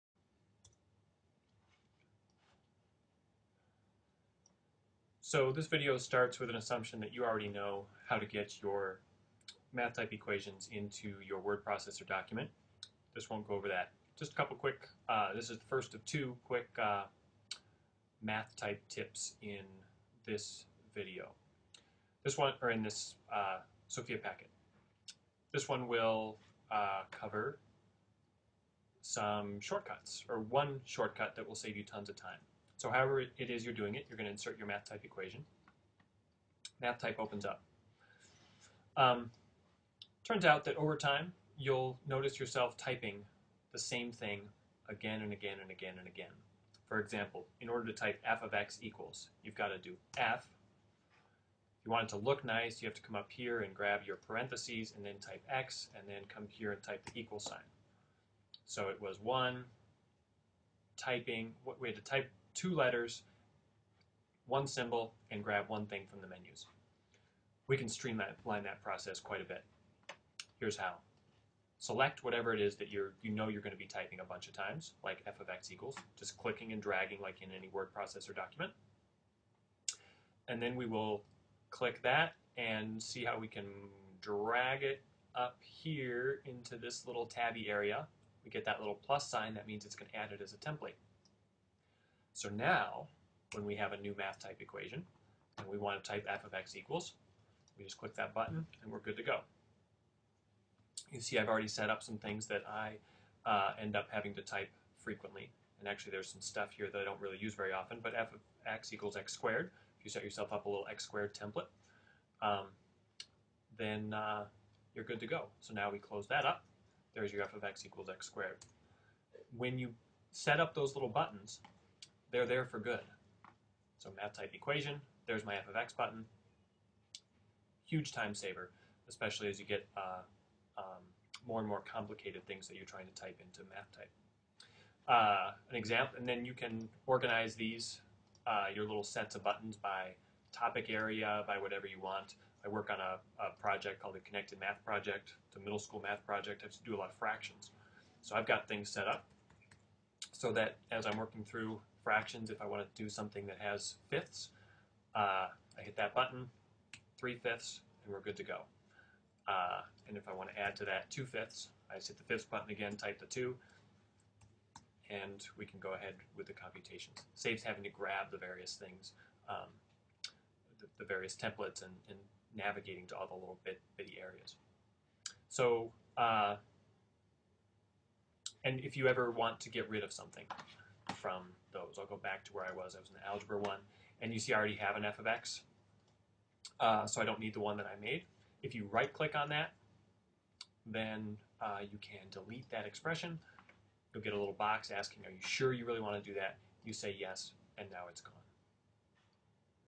MathType tricks #2 Tutorial Learning
Saving expressions as buttons (a time-saving trick as good as copy-and-paste!) This video demonstrates saving frequently used expressions as buttons in MathType.